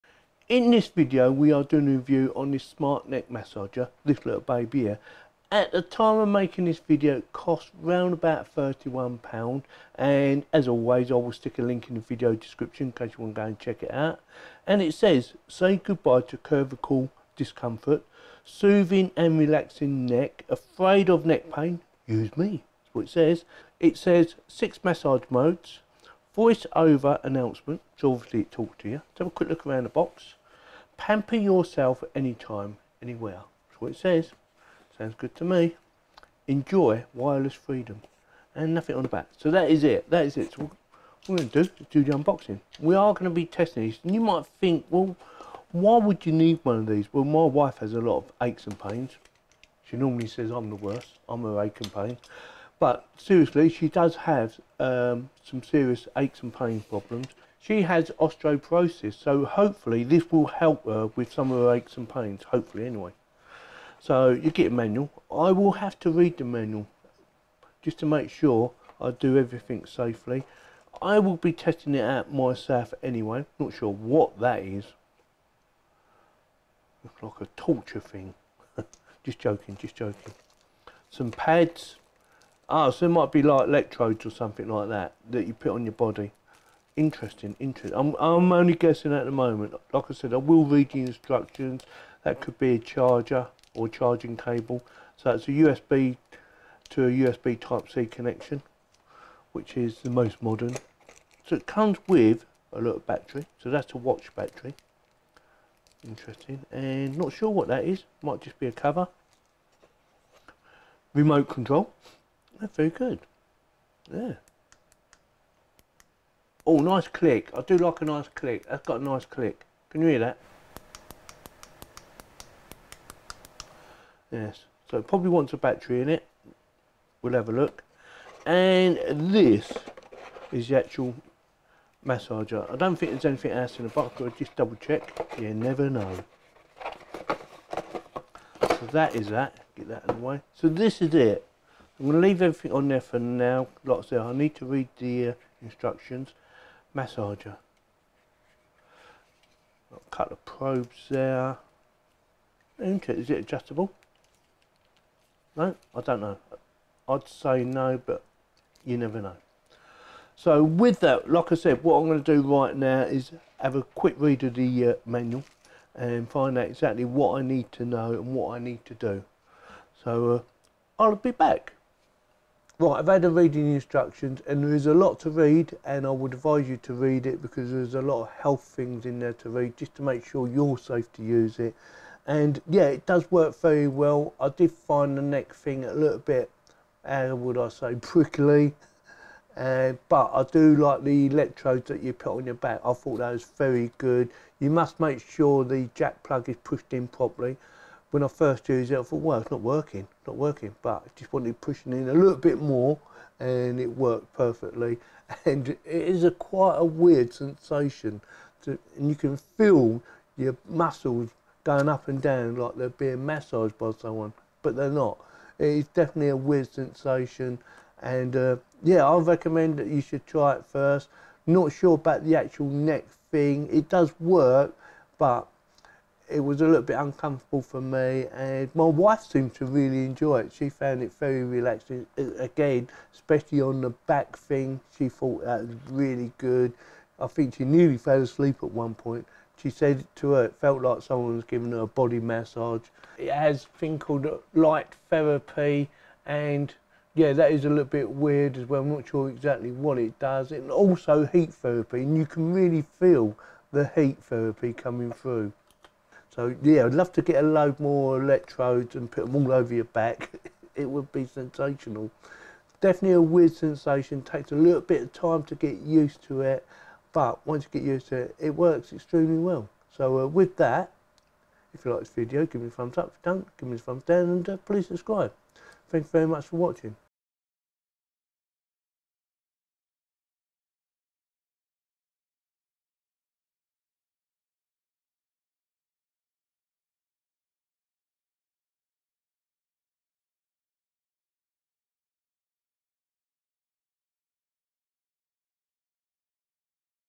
Neck Massager, Unboxing and Review
Neck Massager, Electric Pulse Neck Massager for Pain Relief
Intelligent Deep Tissue Cordless Back Massager with Heating Function, Neck and Shoulder Massager with Red Light Therapy, 6 Modes 16 Levels
Links (paid)
UK Global